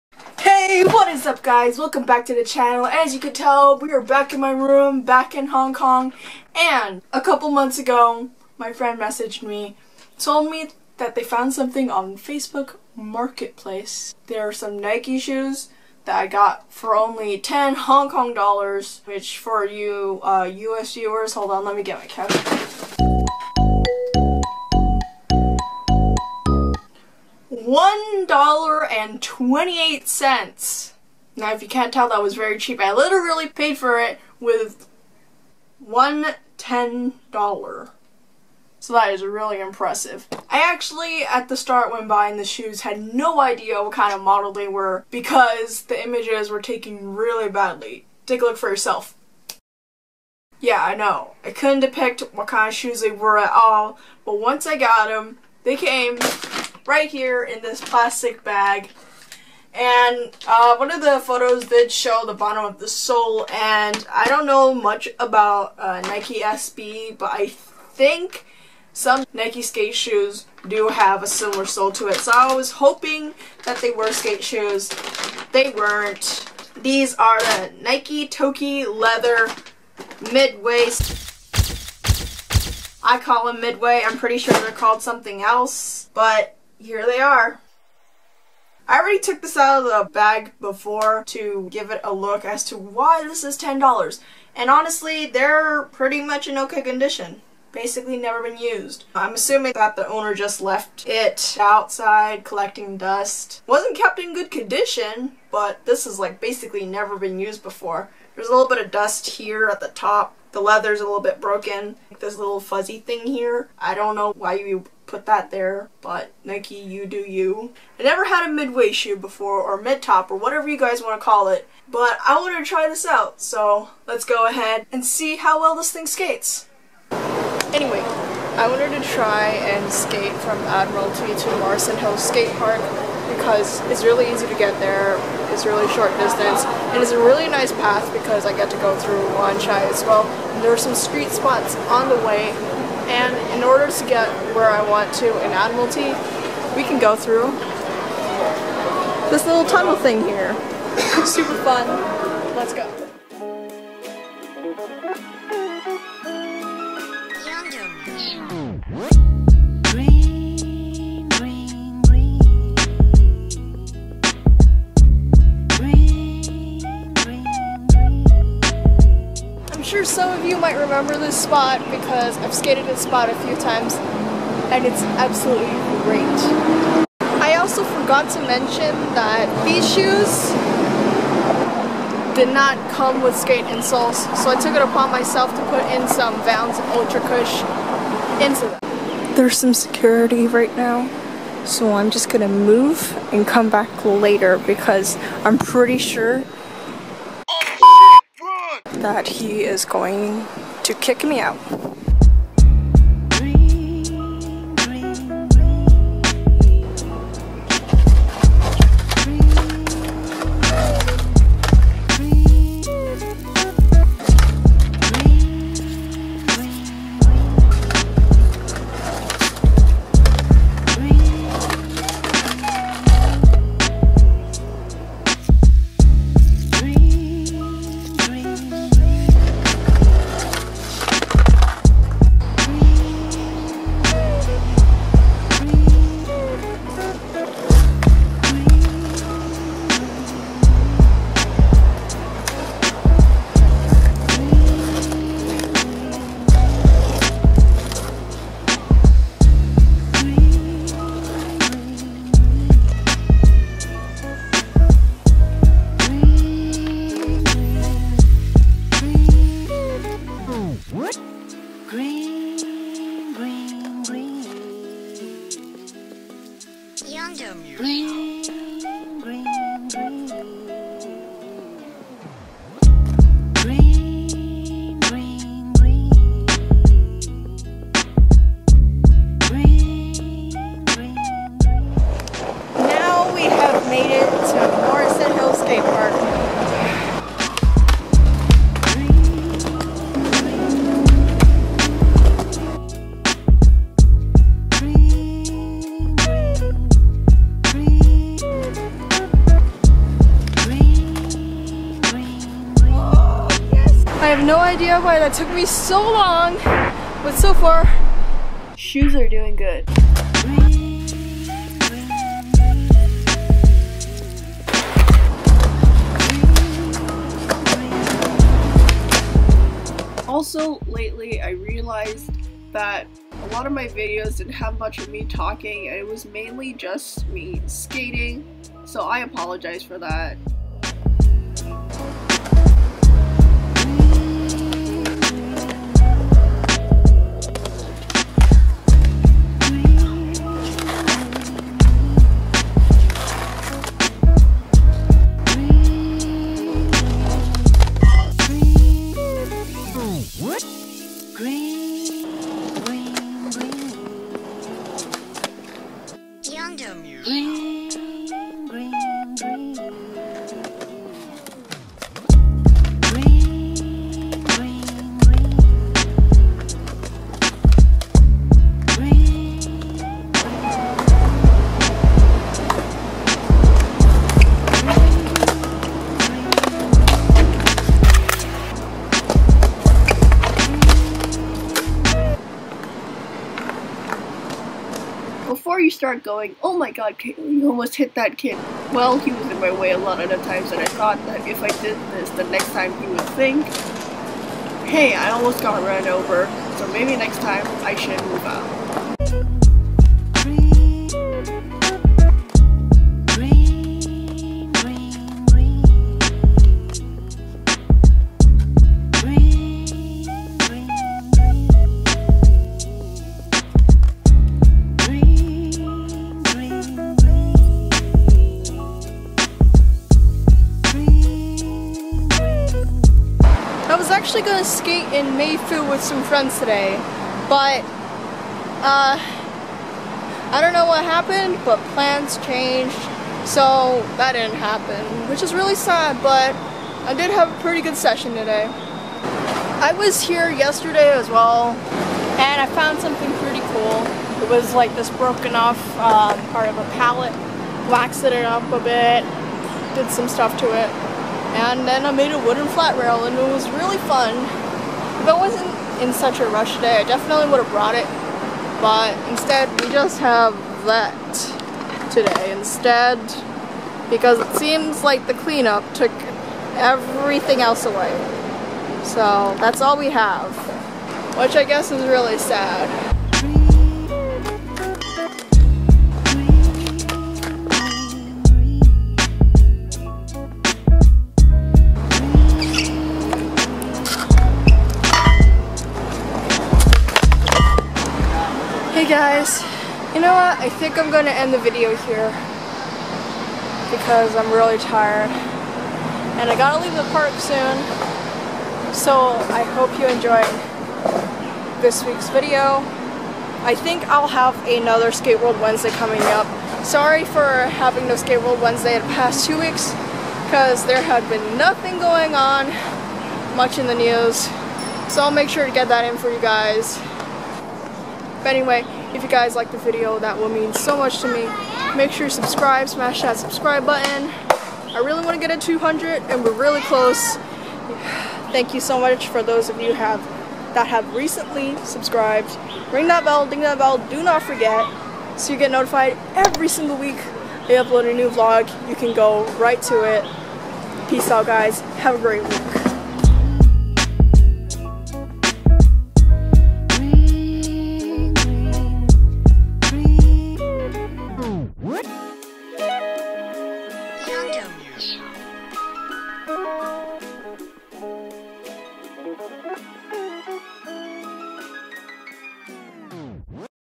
CHEAPEST SKATE SHOES ON THE INTERNET?
Hi my name is Caitlyn Wong and this is my skateboard vlog, hope you enjoy! :)

I may have just found the cheapest yet functional skate shoes!

psssst they aren't real skate shoes, they're the nike took mids but they function just as well, trust me i tested it out in this video